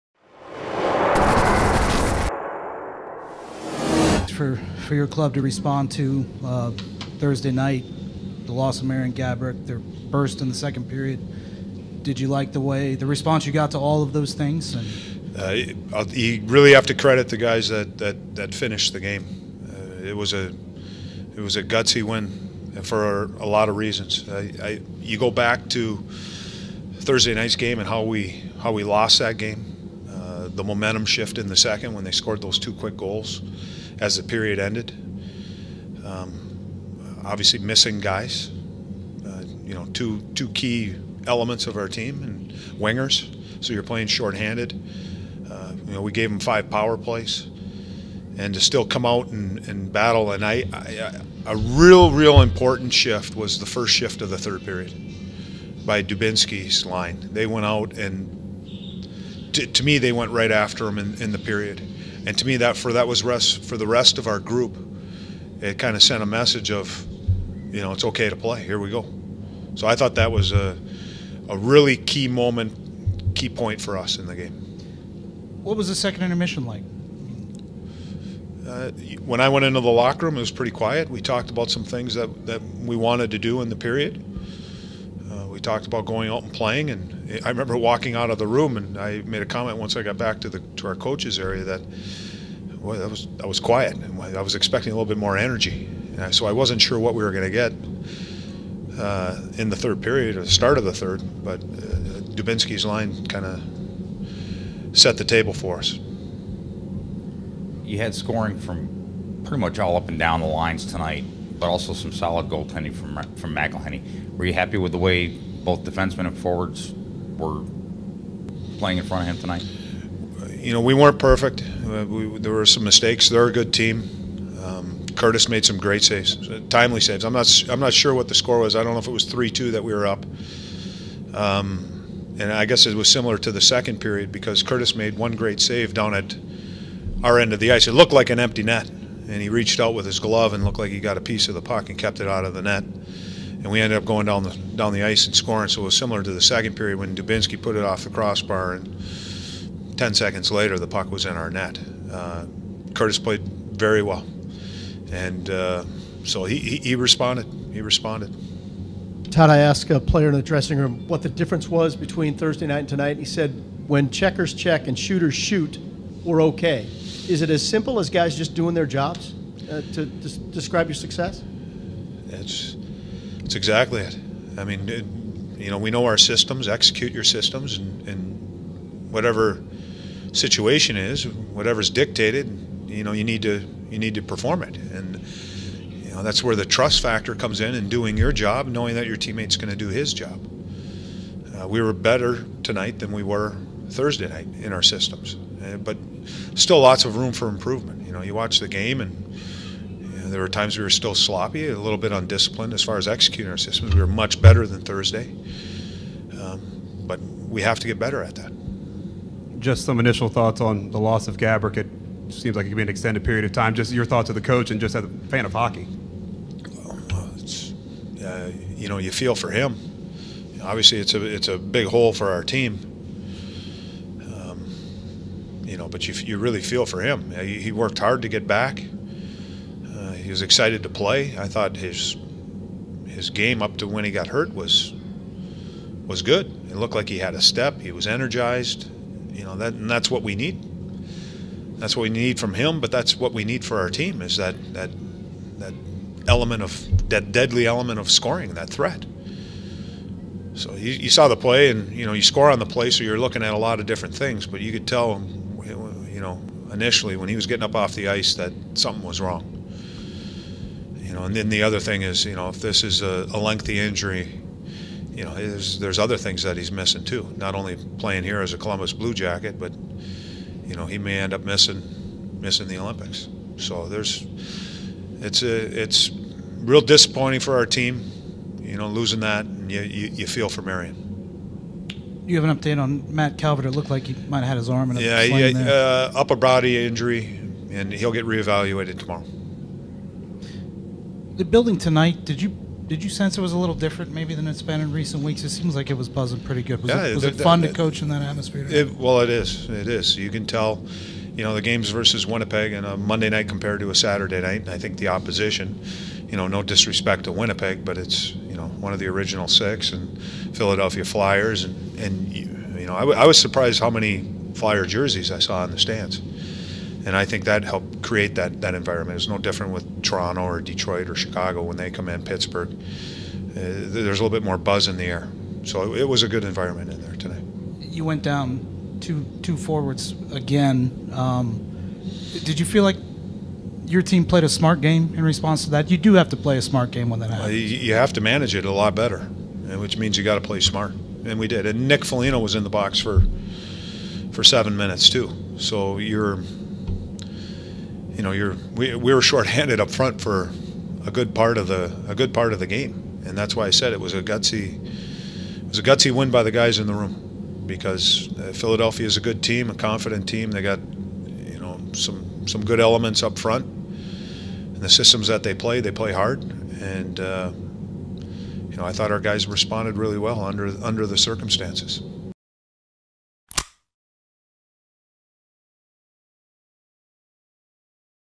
Post-Game Interview: Coach Richards
Coach Richards post-game interview after the Blue Jackets beat the Flyers 6-3 at NWA.